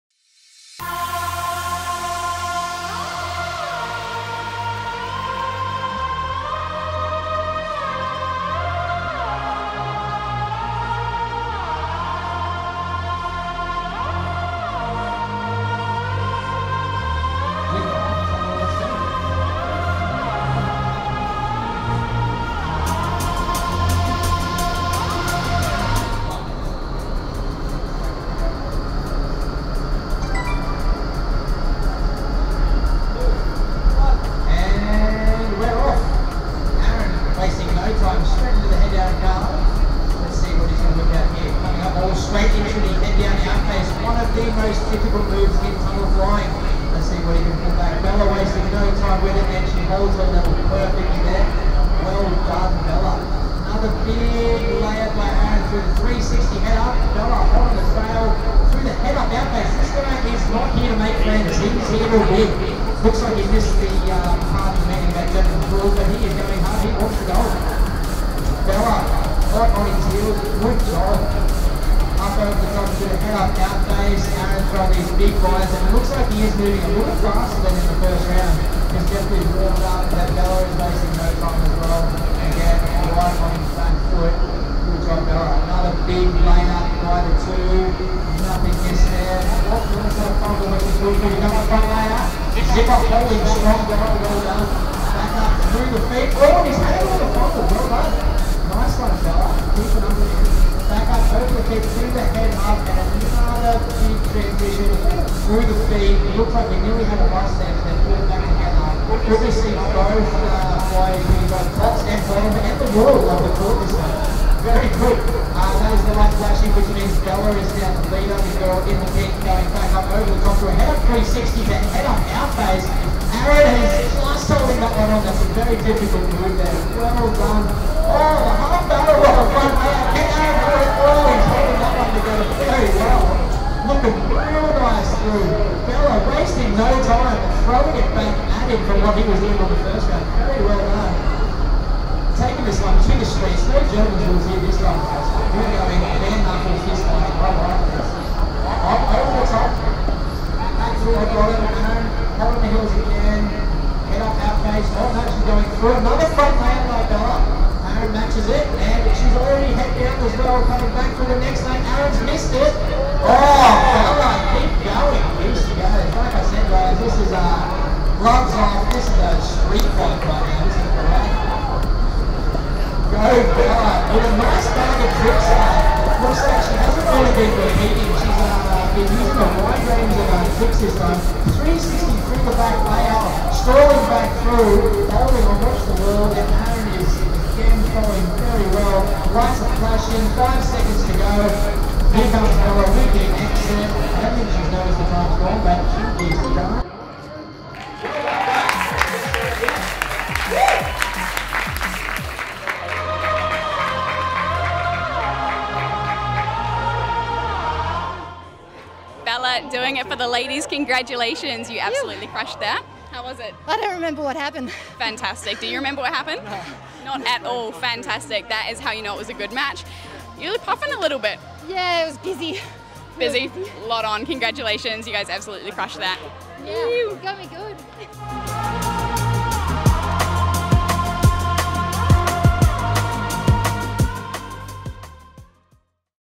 Bella vs Aaron - Body piloting championships
BPC #1 Match between Bella Smart and Aaron Niel

Tunnel - Ifly Downunder, Sydney Australia